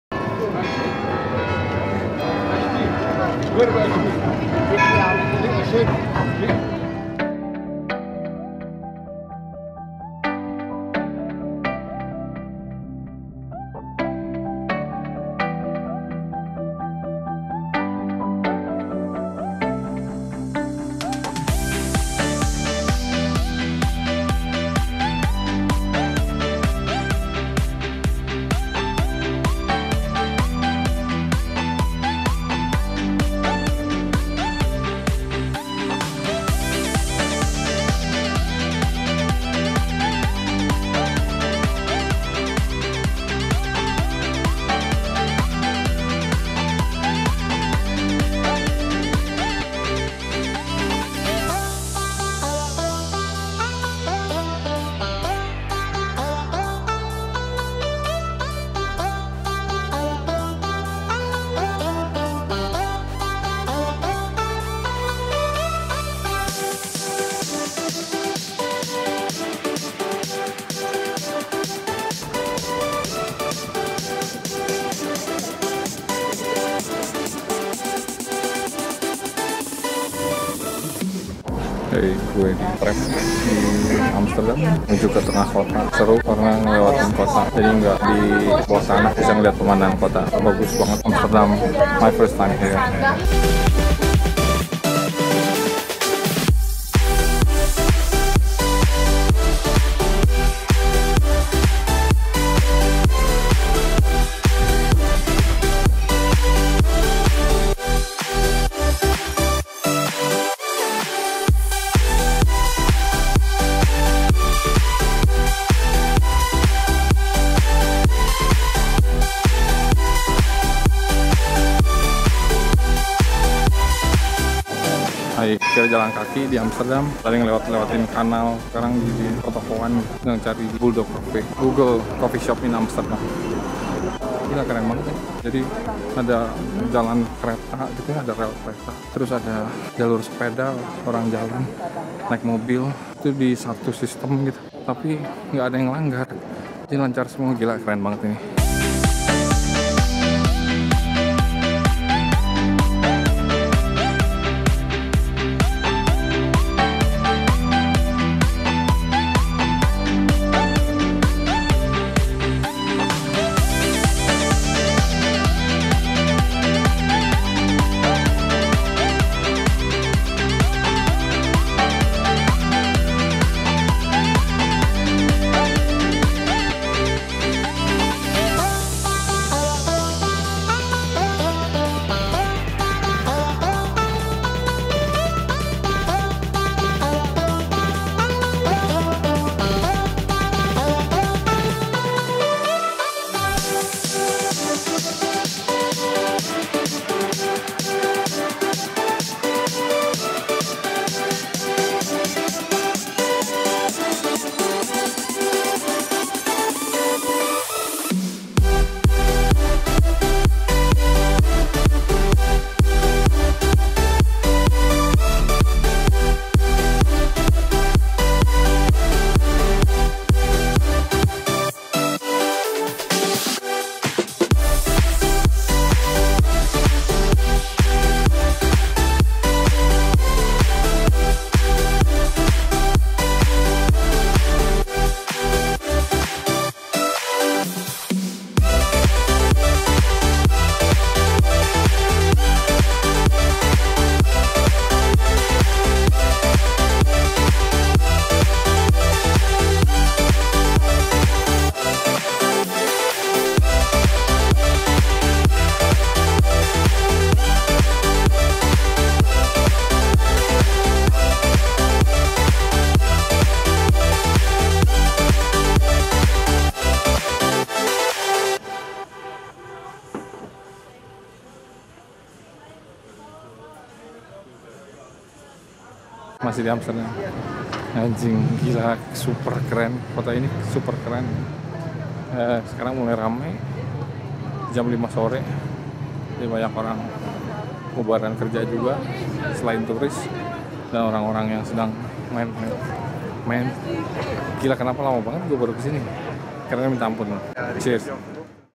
Warung Kopi di Amsterdam
Vlog #38

Jalan-jalan mencari warung kopi di Amsterdam.